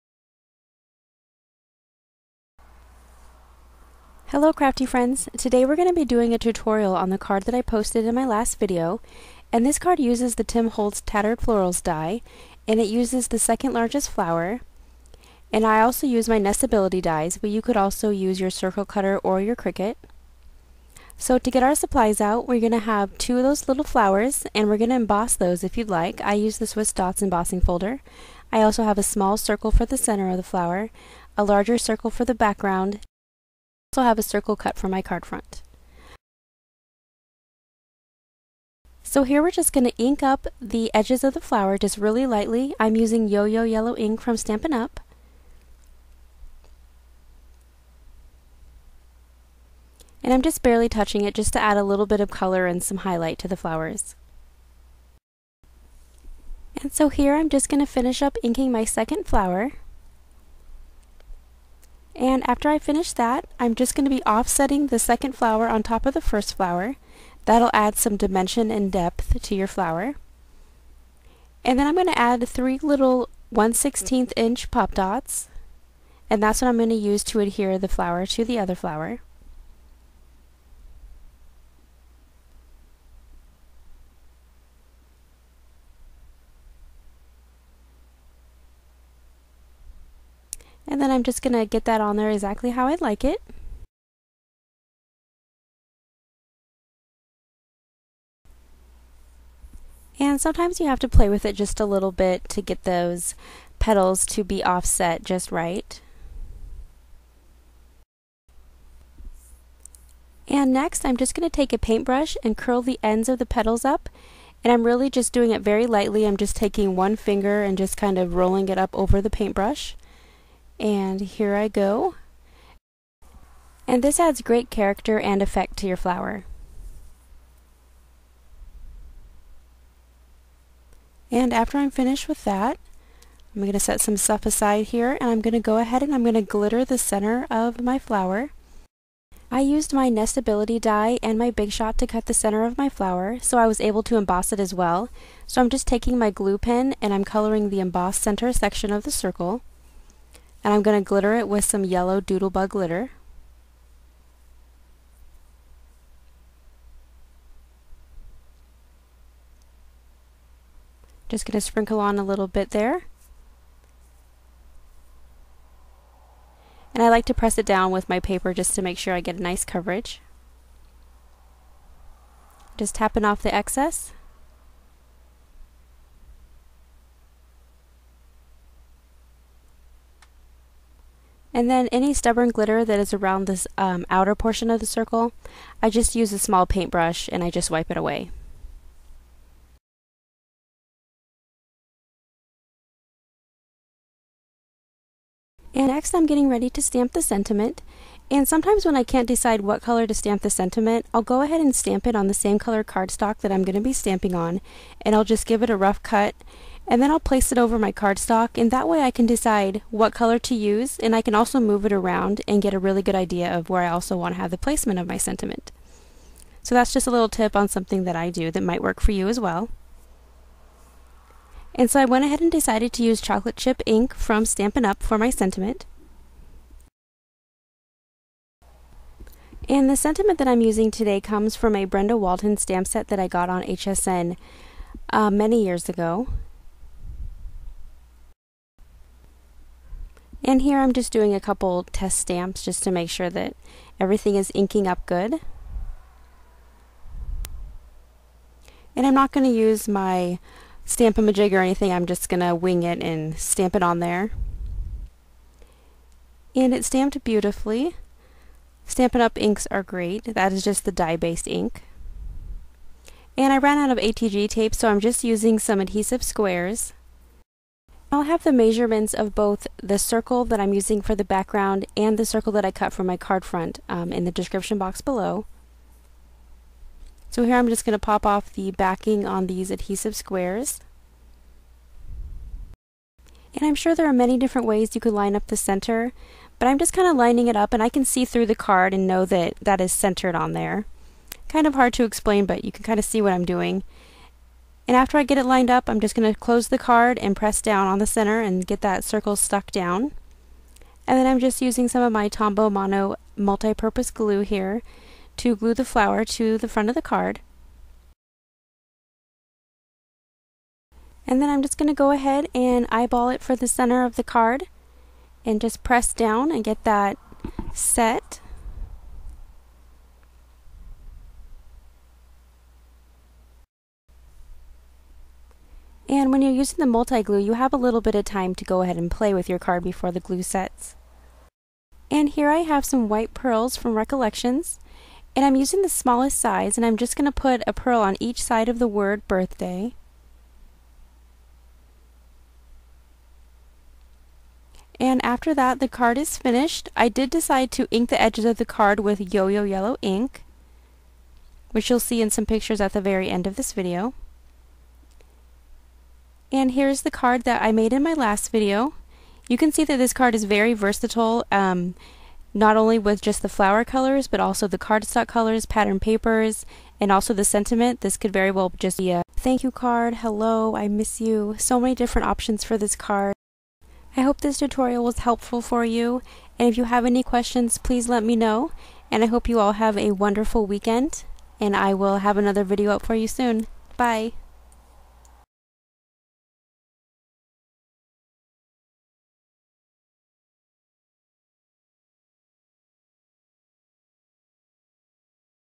Tattered Daisy Card - TUTORIAL
Card tutorial using the Tattered Florals Die from Tim Hotlz.
Circle on card front measures - 3.25
Patterned paper circle measures - 3.50